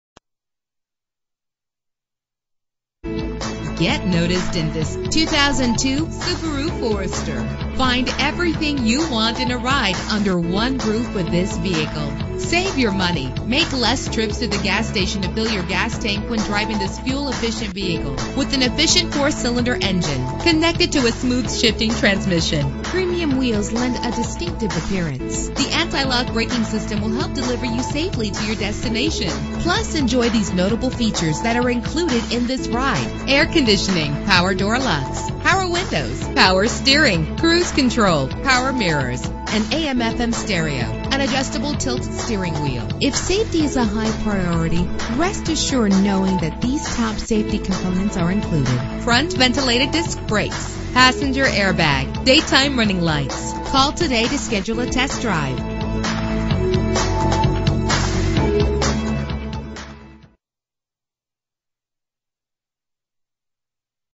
2002 Subaru Forester - Longmont CO
Year: 2002
Make: Subaru
Model: Forester
Trim: L
Engine: 2.5 liter h4 16 valve
Transmission:
Color: Sedona Red Pearl
Mileage: 58365
Address:
1005 Ken Pratt Blvd
Longmont, CO